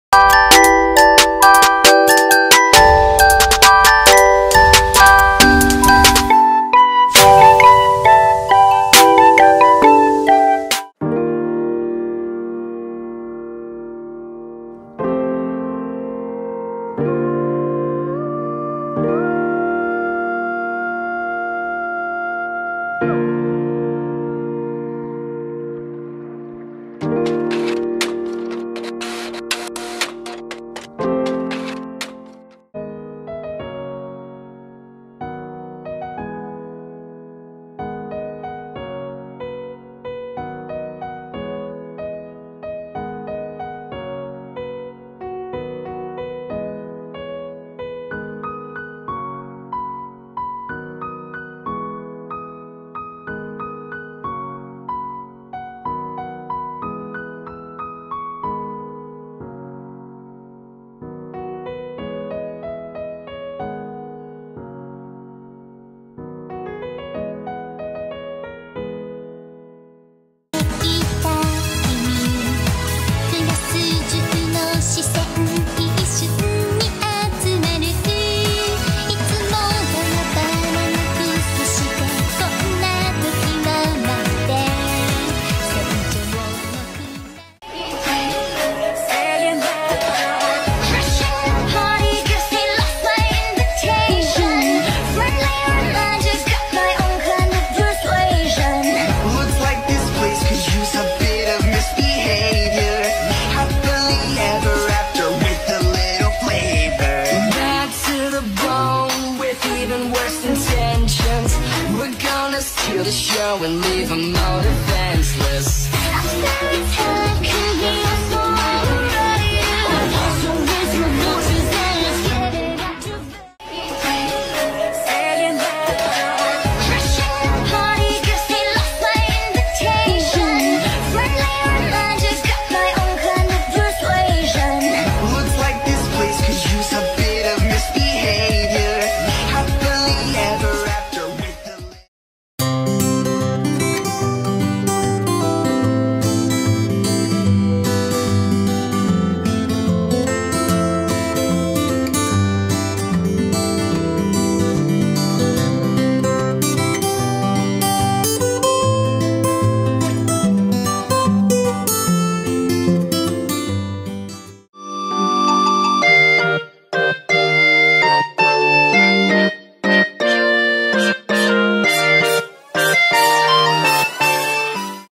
Abnormal Creature Ep 9 (Finalé!!)
It seems as if the the two girls have saved everyone and everything else. I guess things have gone really well for them.